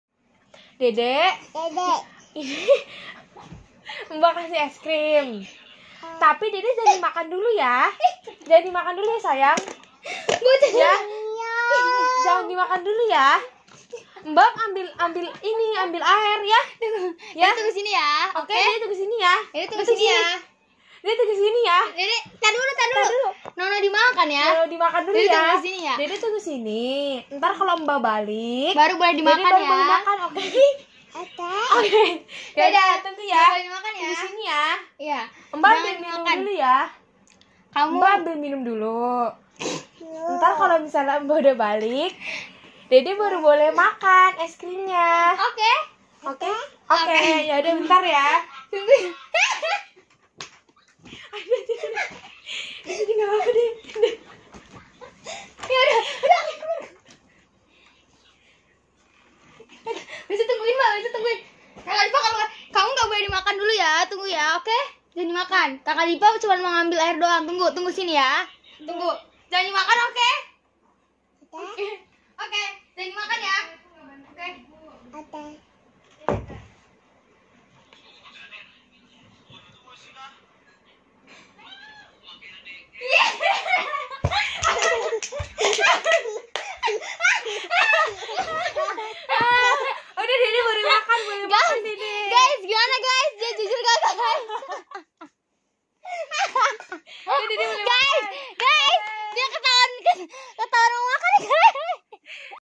Test Kejujuran Bayi
Halloo.. Assalamu'alaikum temen temen.. Kali ini Amih mau berbagi video tentang test kejujuran nih.. Hayohh dibulan puasa g boleh bohong yaa.. Kalo ketahuan bohong bagaimana?

Nahh, Adik Gendhis ini sudah diberi pesan oleh kakaknya.. Tapi kokkkk?? Adek Gendhis ini maaalaaahhh?

Yukk nonton aja keseruannya.. Gimana sih akhir video ini?